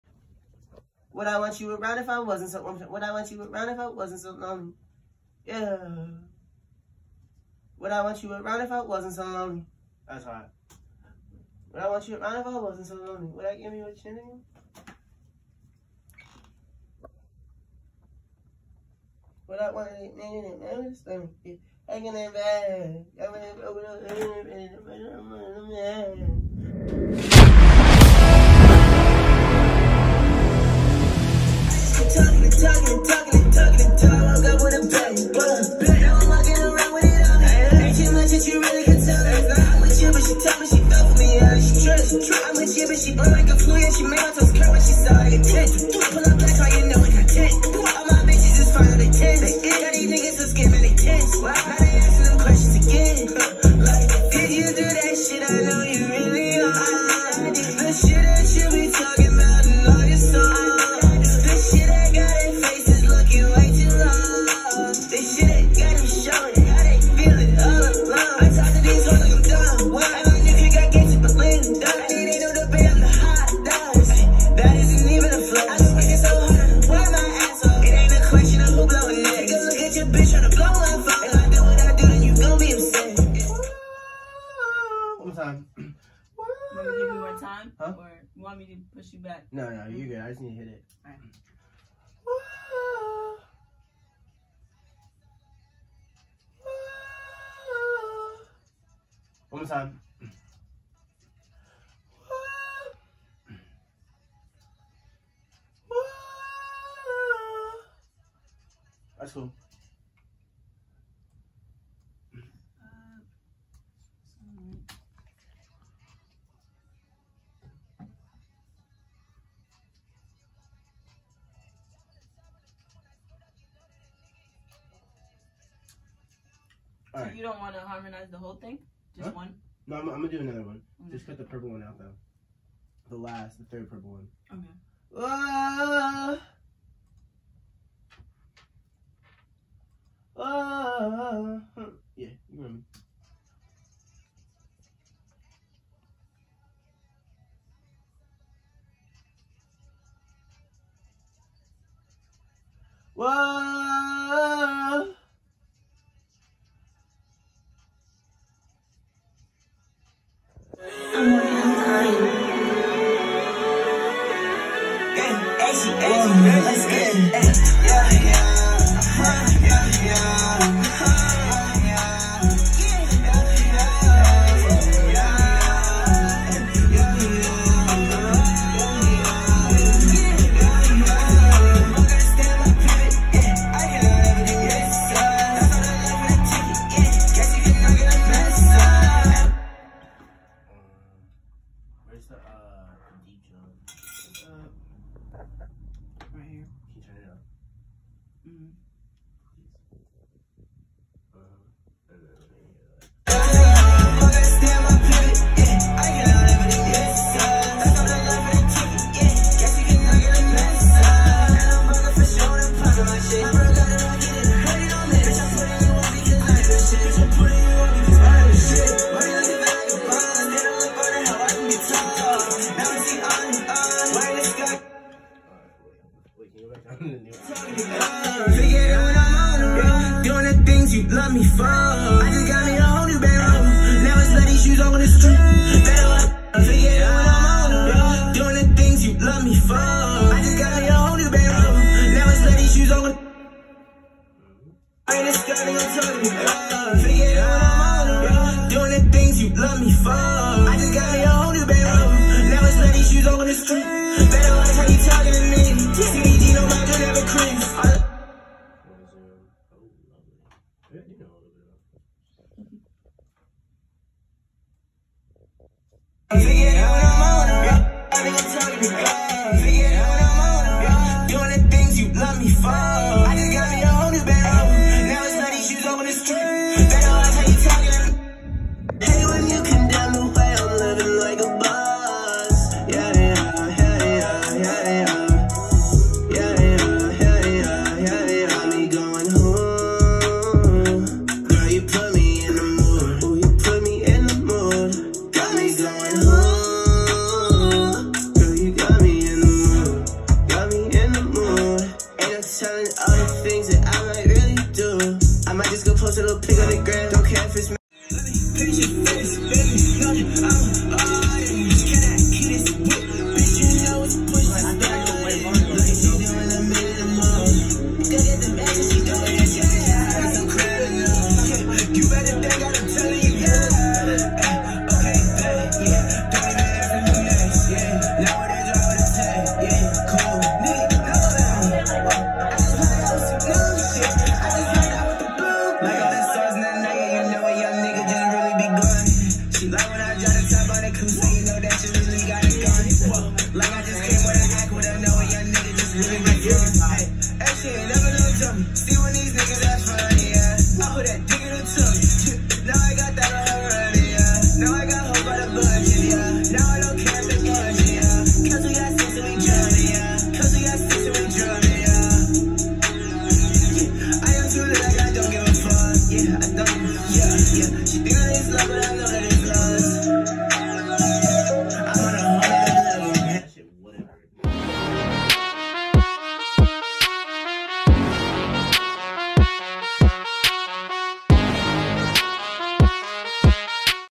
NxG VLOG STUDIO SESSION
NXG SOCIAL

FOOTAGE OF PHILLY RAPPER NXG STUDIO SESSION 🔥